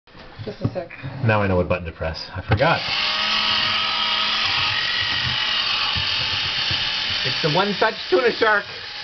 Stupid Sharky Can Opener...
My sister has the coolest can opener going, see it per-form